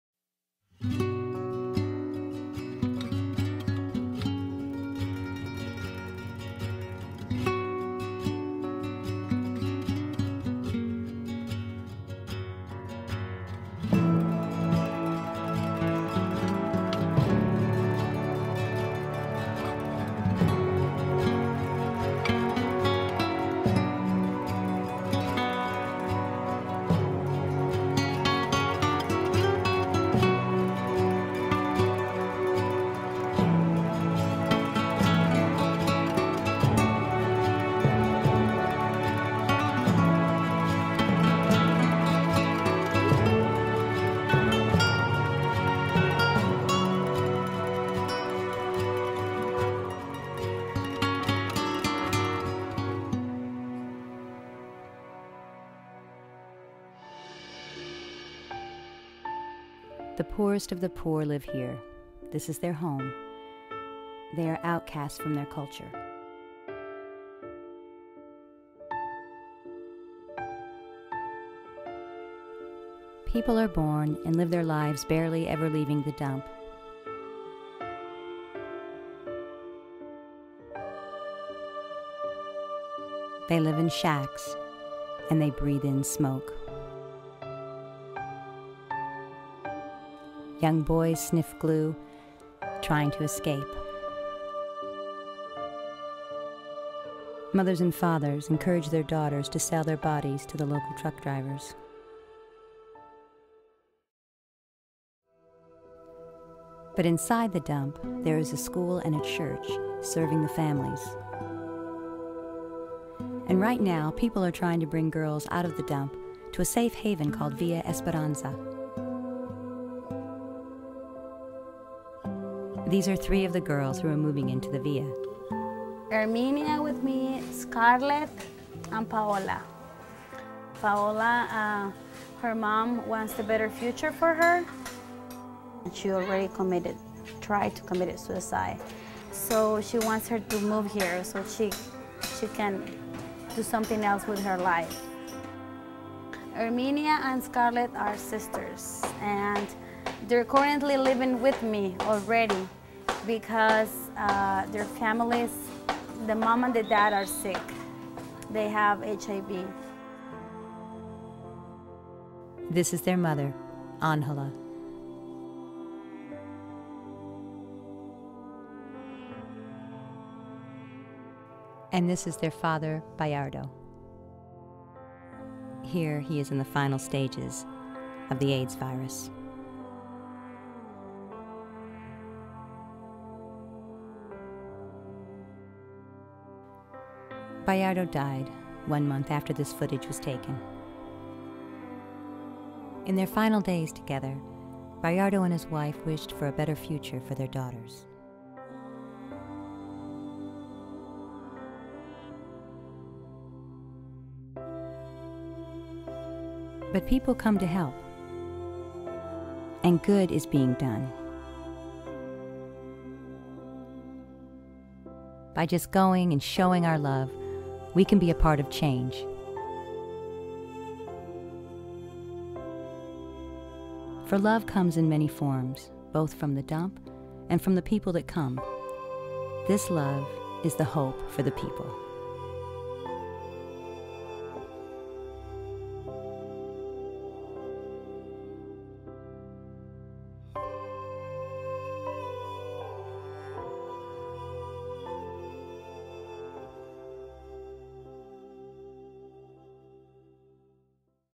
La Chureca
In Managua, Nicaragua there is a place called La Chureca. It is a garbage dump where Nicaragua's poorest people live. This is a short piece showing a small part of their lives. Music by Sam Martin, Narrated by Liz Martin